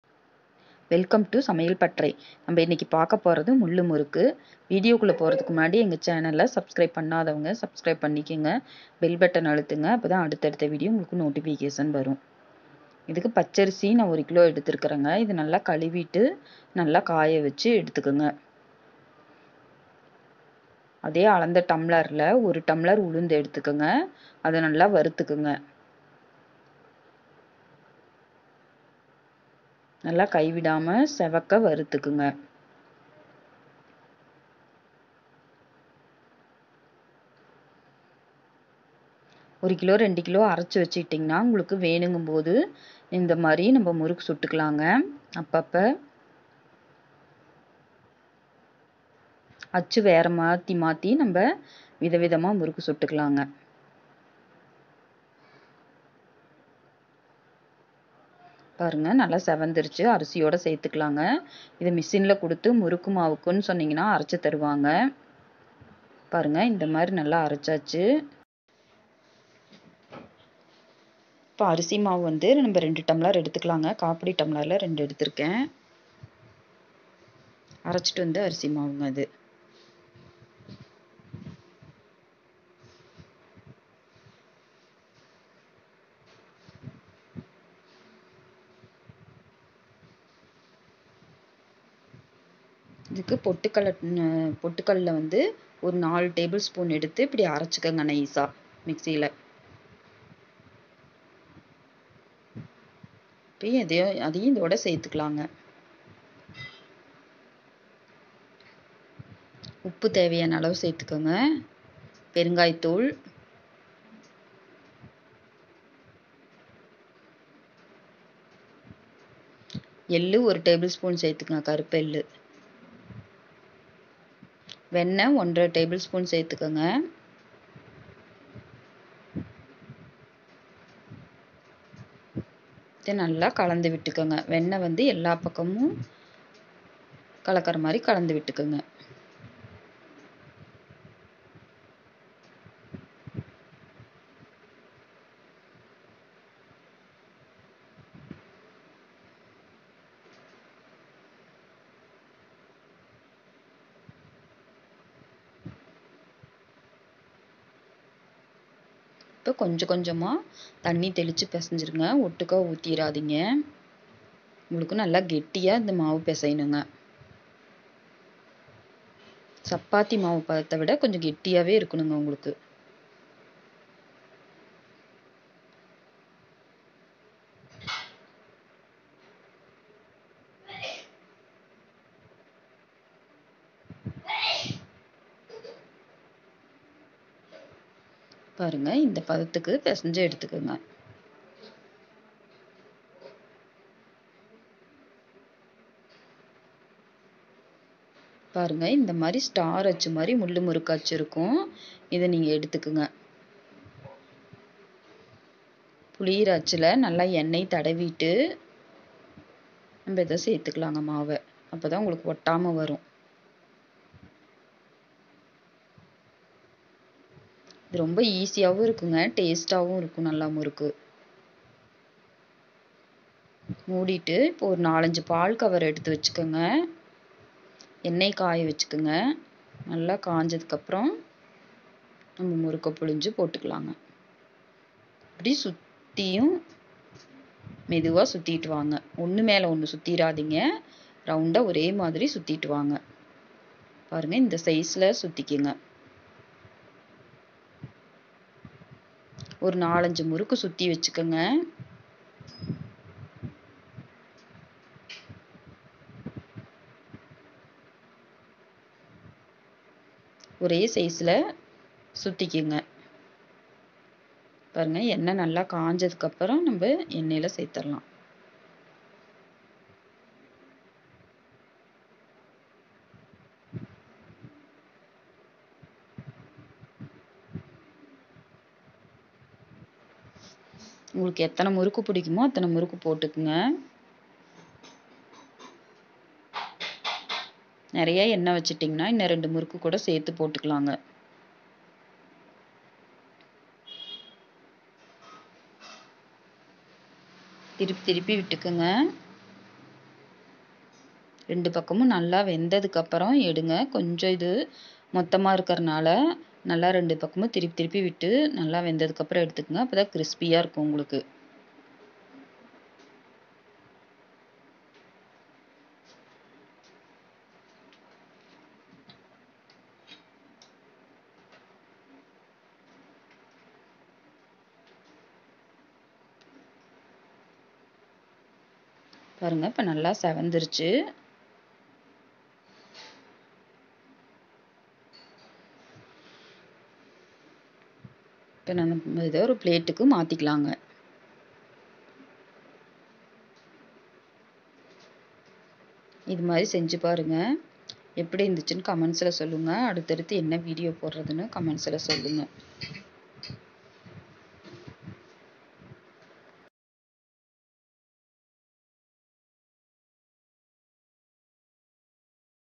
Mullu Murukku in Tamil | Diwali SPL recipes | Mullu Thenkuzhal | முள்ளு முறுக்கு
Mullu Thenkuzhal is a famous snacks recipe can be prepared easily.This mullu thenkuzhal tastes so delicious as it's name itself denoting it's taste. For this thenkuzhal Murukku recipe no need to use grinder, we need to prepare rice and urad dal powder in the ratio of 4:1 only urad dal need to dry roast. once you prepared this Murukku powder we can do Murukku whenever we want. Try this thenkuzhal Murukku in your home and surprise your family members and share your experience with us.